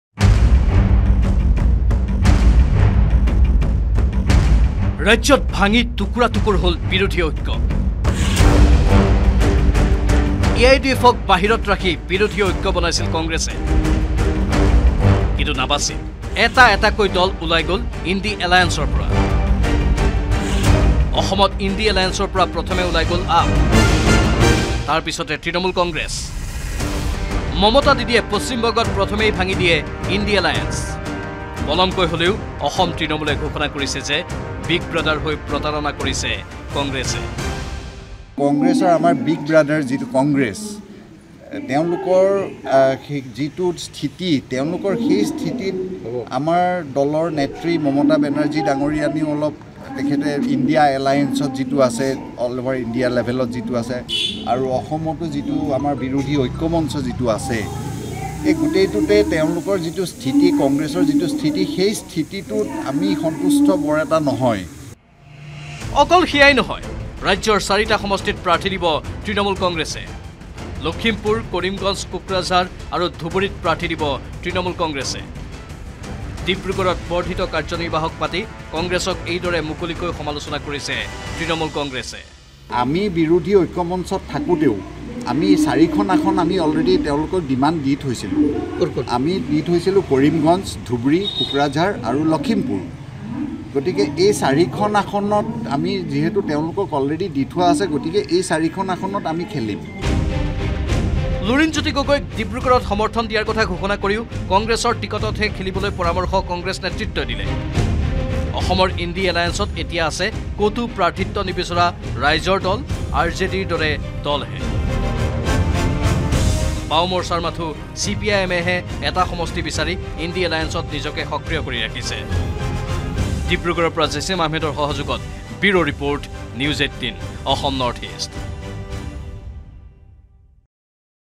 Opposition News : নিৰ্বাচনৰ পূৰ্বে অসমত তচনচ হৈছে বিৰোধী ঐক্য | N18V
Opposition News | নিৰ্বাচনৰ পূৰ্বে অসমত তচনচ হৈছে বিৰোধী ঐক্য। কংগ্ৰেছে আভ্যন্তৰীণ সংঘাত মোকাবিলা কৰি থাকোতেই আঁতৰি গৈছে ইটোৰ পিছত সিটো দল। আপে  তিনিটা সমষ্টিত প্ৰাৰ্থী ঘোষণা কৰাৰ পিছত চাৰিটাকৈ সমষ্টিত নিজাববীয়া প্ৰাৰ্থিত্ব দিব তৃণমূল কংগ্ৰেছে।